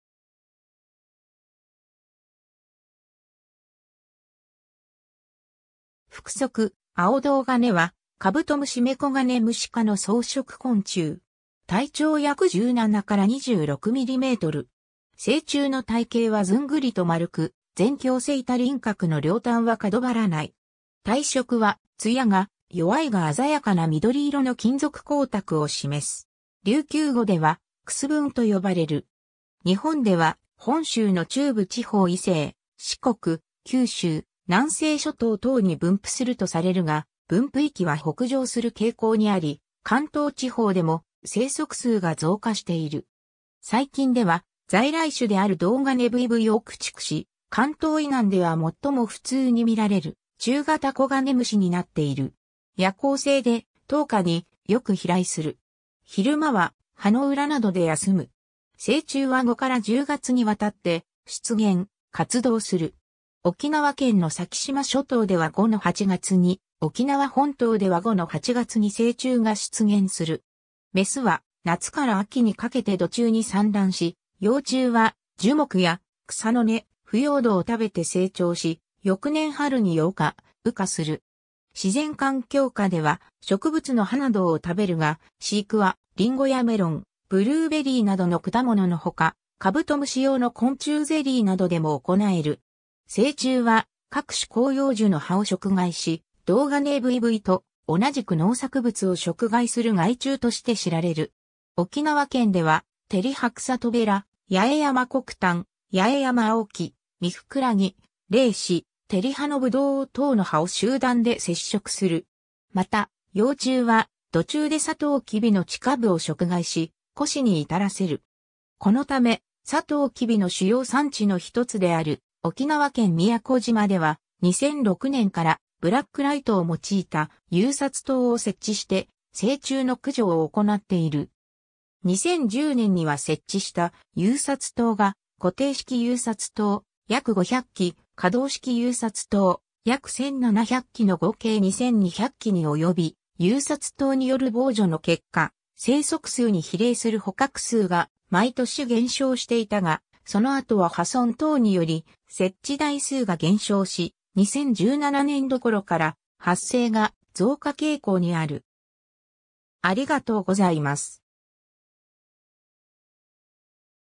アオドウガネ
腹側 アオドウガネ（青銅鉦、Anomala albopilosa）は、甲虫目コガネムシ科の草食昆虫。
 体長約 17 - 26mm。
成虫の体型はずんぐりと丸く、前胸背板輪廓の両端は角張らない。
体色は、艶が弱いが鮮やかな緑色の金属光沢を示す。
琉球語（沖縄方言）ではクスブンと呼ばれる。
 日本では本州の中部地方以西、四国、九州、南西諸島等に分布するとされるが、分布域は北上する傾向にあり、関東地方でも生息数が増加している。
最近では在来種であるドウガネブイブイを駆逐し、関東以南では最も普通に見られる中型コガネムシになっている。
 夜行性で、灯火によく飛来する。
昼間は葉の裏などで休む。
 成虫は5-10月にわたって出現、活動する。
沖縄県の先島諸島では5-8月に、沖縄本島では5-8月に成虫が出現する。
メスは夏から秋にかけて土中に産卵し、幼虫は樹木や草の根、腐葉土を食べて成長し、翌年春に蛹化、羽化する。
 自然環境下では植物の葉などを食べるが、飼育はリンゴやメロン、ブルーベリーなどの果物のほか、カブトムシ用の昆虫ゼリーなどでも行える。
 成虫は各種広葉樹の葉を食害し、ドウガネブイブイと同じく農作物を食害する害虫として知られる。
沖縄県では、テリハクサトベラ、ヤエヤマコクタン（リュウキュウコクタン）、ヤエヤマアオキ(ノニ)、ミフクラギ（オキナワキョウチクトウ）、レイシ、テリハノブドウ等の葉を集団で摂食する。
 また、幼虫は土中でサトウキビの地下部を食害し、枯死に至らせる。
このため、サトウキビの主要産地のひとつである沖縄県宮古島では、2006年からブラックライトを用いた誘殺灯を設置して成虫の駆除を行っている。
2010年には設置した誘殺灯が固定式誘殺灯約500基、可動式誘殺灯約1,700基の合計2,200基に及び、誘殺灯による防除の結果、生息数に比例する捕獲数が毎年減少していたが、その後は破損等により設置台数が減少し、2017年度頃から発生が増加傾向にある。